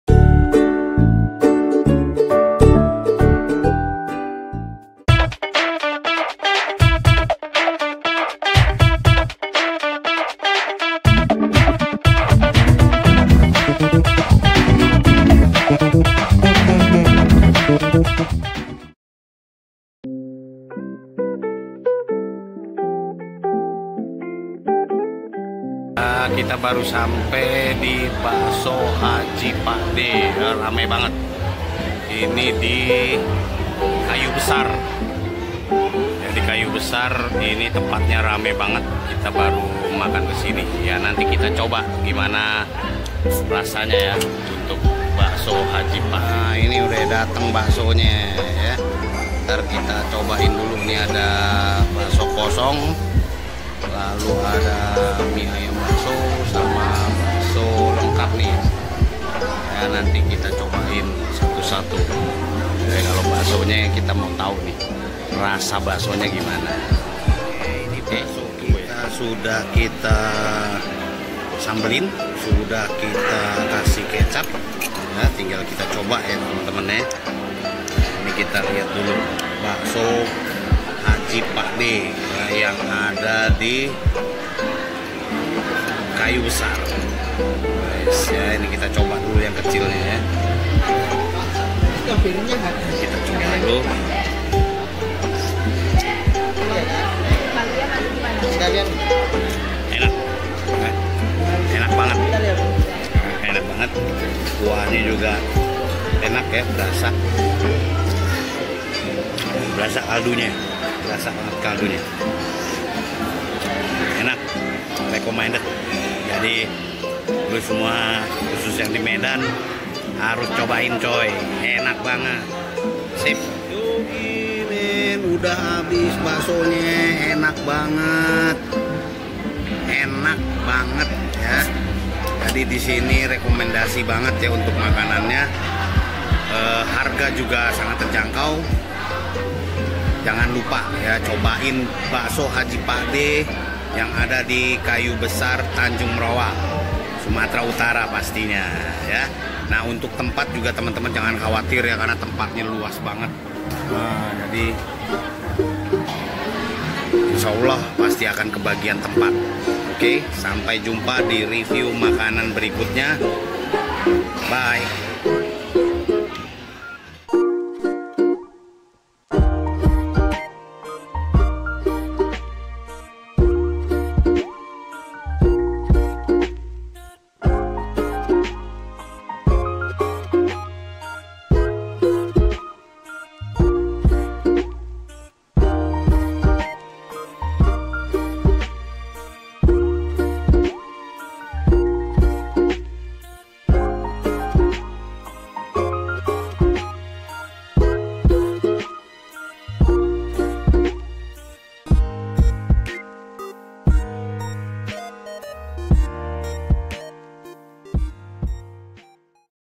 Bakso Enak " Bakso Haji Pakde " di Tanjung Morawa Sumut
Bakso Haji Pakde di Tanjung Morawa Sumut enak banget, tempatnya bersih, nyaman, harga terjangkau dan banyak banget Menu yang berhubungan dengan Bakso
Datang langsung biar tau enaknya kayak apa
Follow Ig : @botakbicarahobby